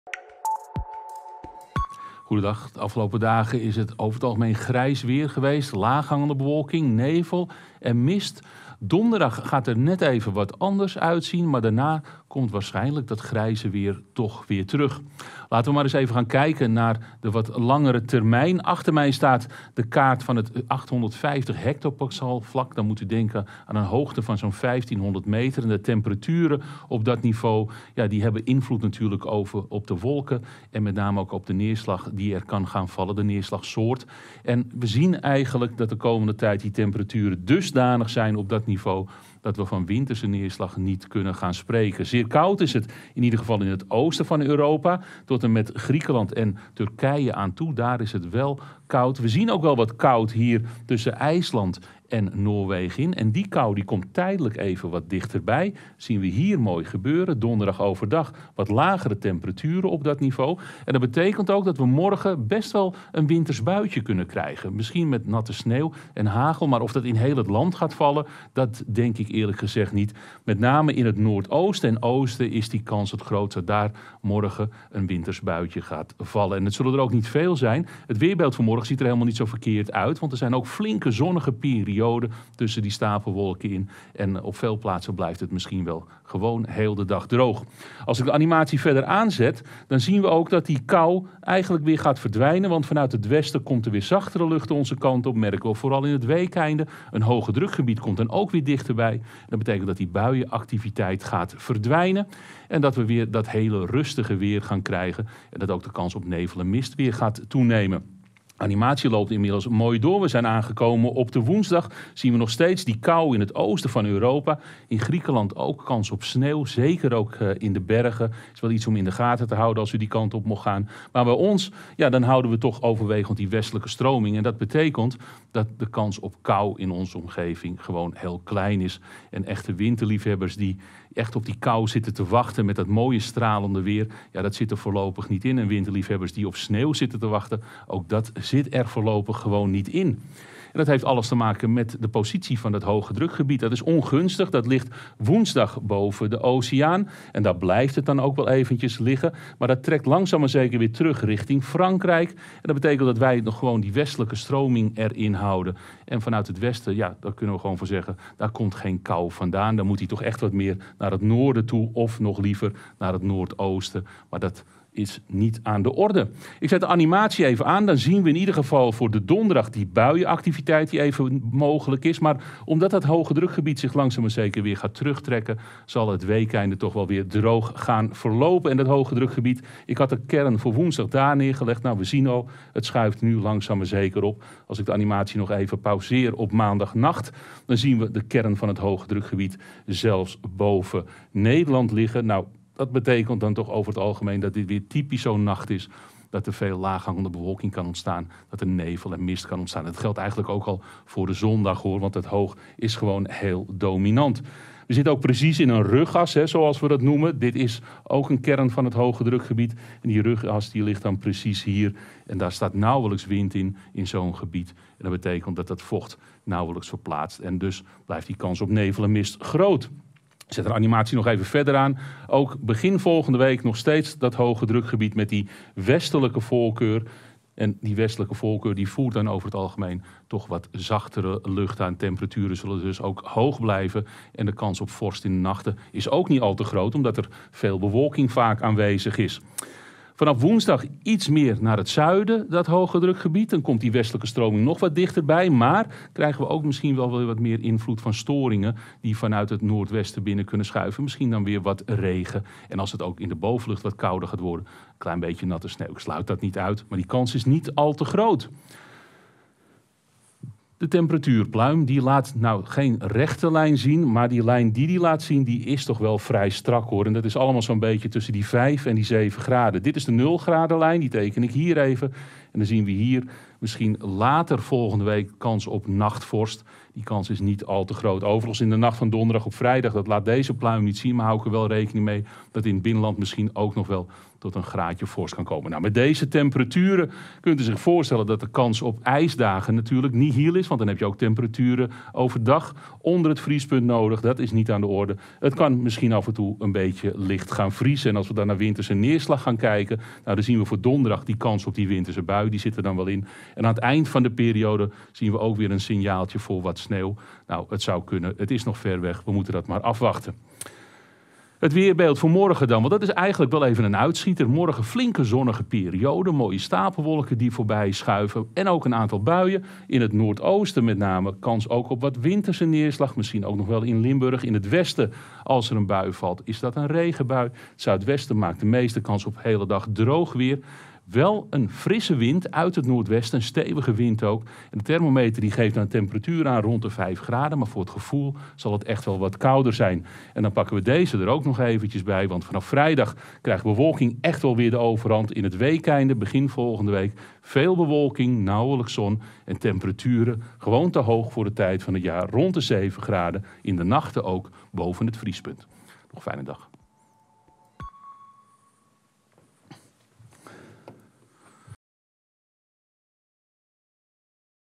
10-daagse: kunnen we nog kouder weer verwachten?
In onze nieuwe 10-daagse video kijken wij naar de weersverwachting op middellange termijn. Wordt het nog kouder komende periode? Raymond Klaassen gaat er dieper op in!